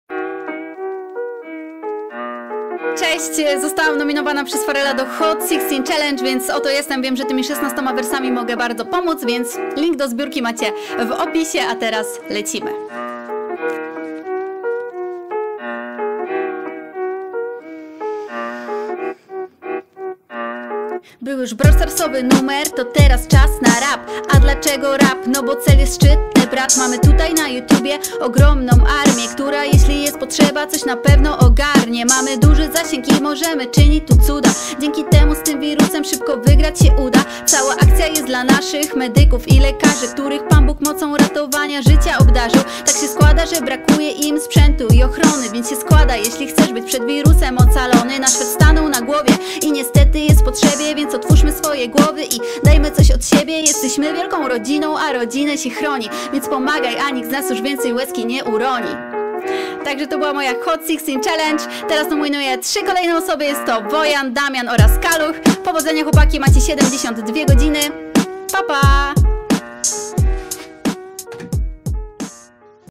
DŻEJDŻEJKA Brzydka16Challenge
Dzięki @FarellYoutube za nominację!
W związku z panującą pandemią, integralną częścią akcji staje się zbiórka charytatywna hot16challenge organizowana wraz z fundacją Siepomaga.pl. Pieniądze w całości zostaną przeznaczone na pomoc lekarzom i szpitalom.